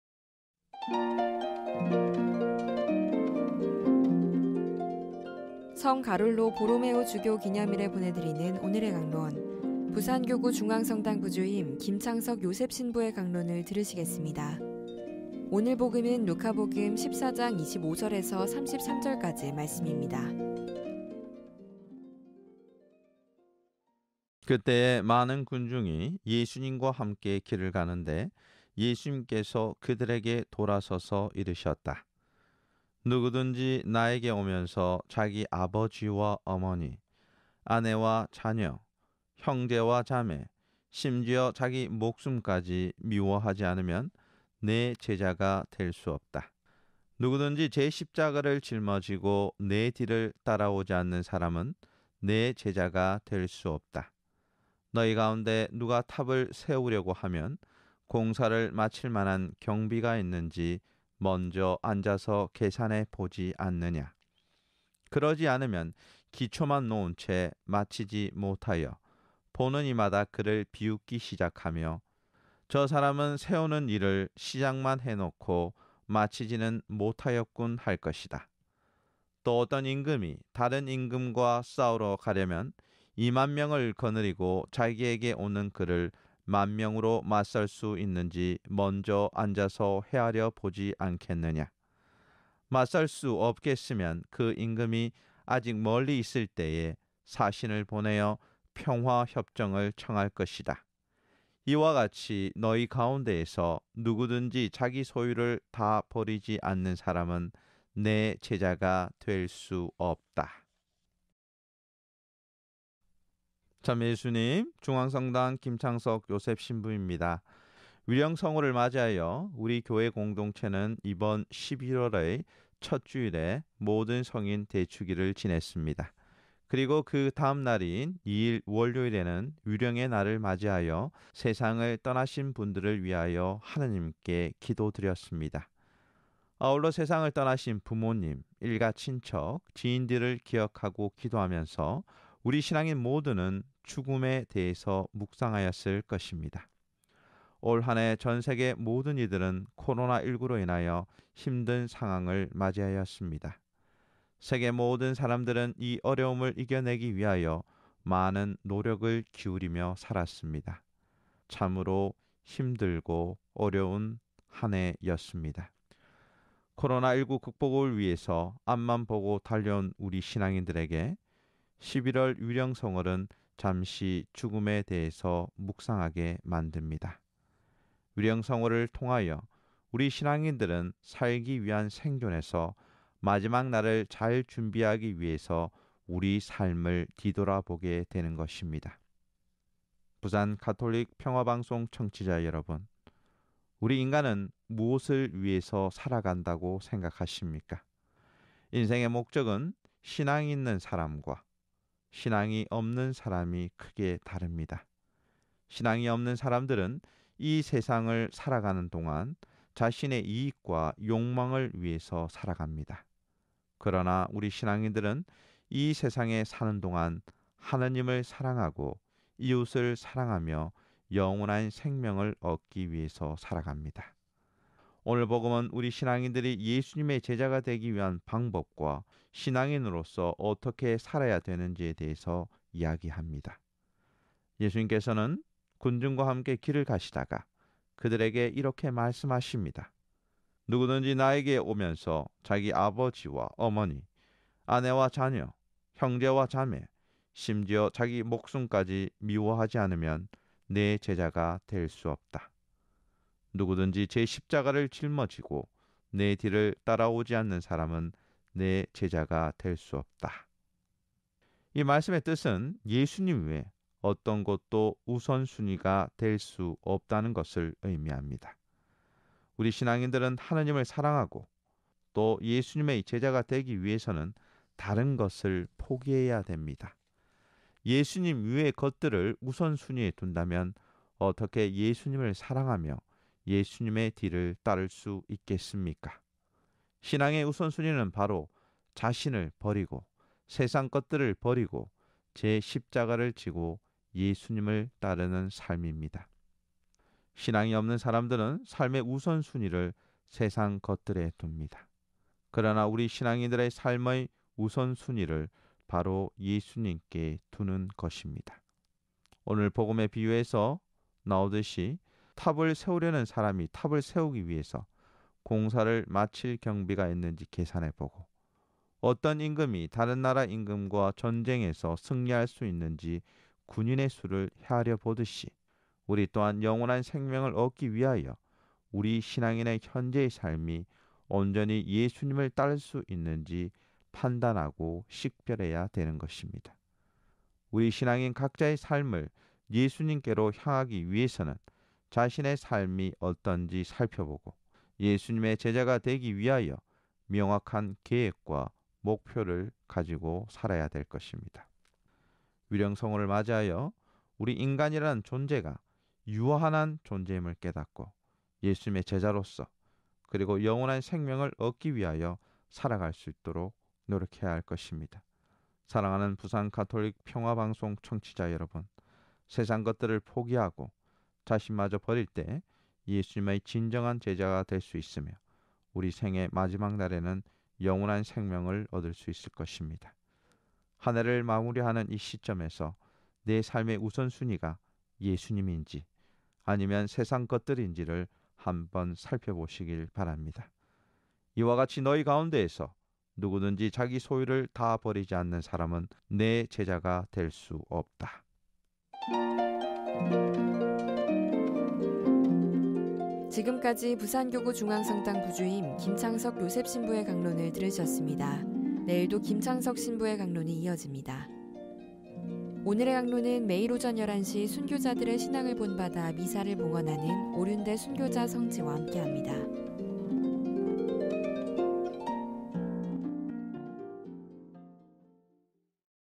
오늘의 강론 (2020.11.04) 중앙성당 부주임 김창석 요셉 신부님
✠ 루카가 전한 거룩한 복음입니다.                               14,25-33
그때에 많은 군중이 예수님과 함께 길을 가는데,
예수님께서 그들에게 돌아서서 이르셨다.
“누구든지 나에게 오면서
자기 아버지와 어머니, 아내와 자녀, 형제와 자매,
심지어 자기 목숨까지 미워하지 않으면, 내 제자가 될 수 없다.
누구든지 제 십자가를 짊어지고 내 뒤를 따라오지 않는 사람은
내 제자가 될 수 없다.
너희 가운데 누가 탑을 세우려고 하면,
공사를 마칠 만한 경비가 있는지 먼저 앉아서 계산해 보지 않느냐?
그러지 않으면 기초만 놓은 채 마치지 못하여,
보는 이마다 그를 비웃기 시작하며,
‘저 사람은 세우는 일을 시작만 해 놓고 마치지는 못하였군.’ 할 것이다.
또 어떤 임금이 다른 임금과 싸우러 가려면,
이만 명을 거느리고 자기에게 오는 그를 만 명으로 맞설 수 있는지
먼저 앉아서 헤아려 보지 않겠느냐?
맞설 수 없겠으면,
그 임금이 아직 멀리 있을 때에 사신을 보내어 평화 협정을 청할 것이다.
이와 같이 너희 가운데에서 누구든지 자기 소유를 다 버리지 않는 사람은